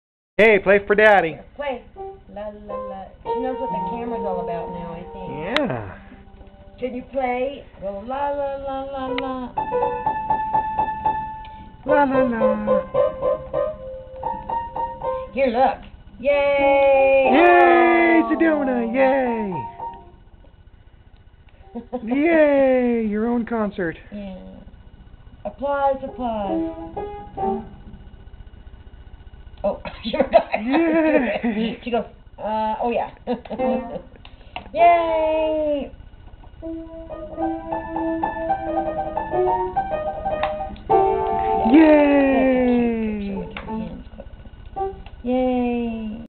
Sedona plays Piano with her Grandma Boender
Sweet Baby Sedona plays the family piano with her Grandma Boender.  She loves to play music and sing and dance!!